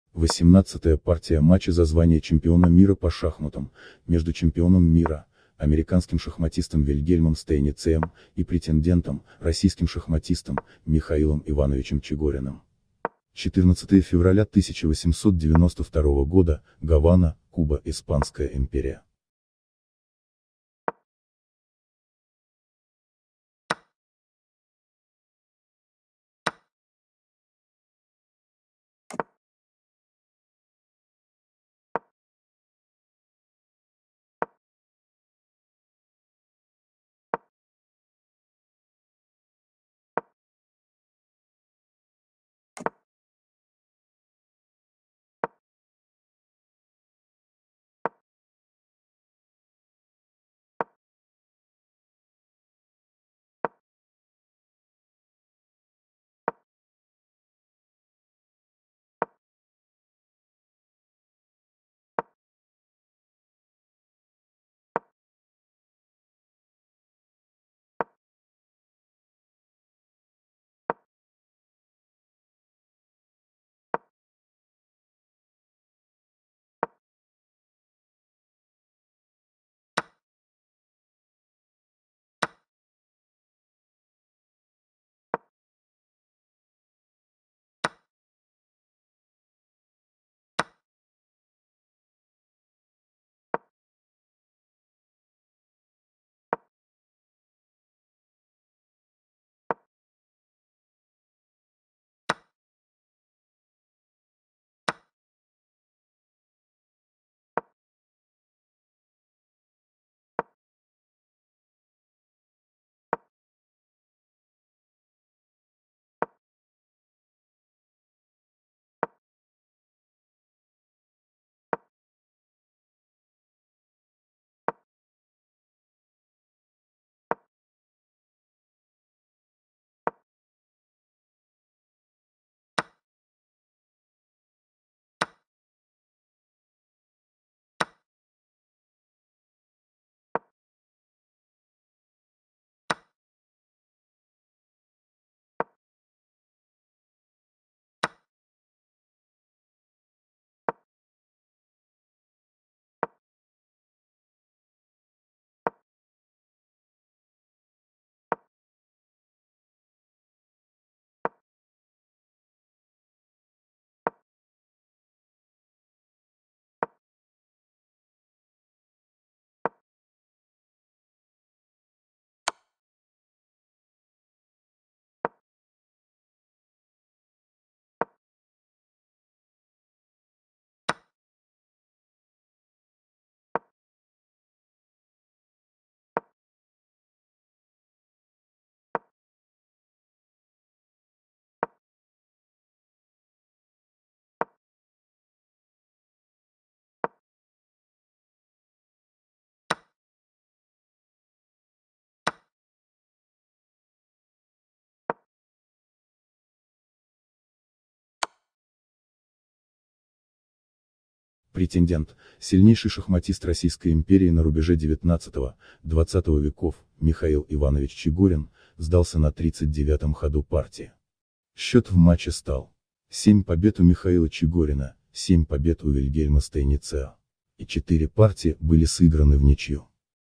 18-я партия Стейниц - Чигорин, матч на первенство мира 1892 года, Гавана. (1-0)
18-я партия матча за звание чемпиона мира по шахматам между чемпионом мира Вильгельмом Стейницем (США) и претендентом Михаилом Ивановичем Чигориным (Российская империя),
14-е февраля 1892 года, Гавана, Куба, Испанская империя.

Голландская защита.

Использованы фото с Wikimedia Commons. Общественное достояние.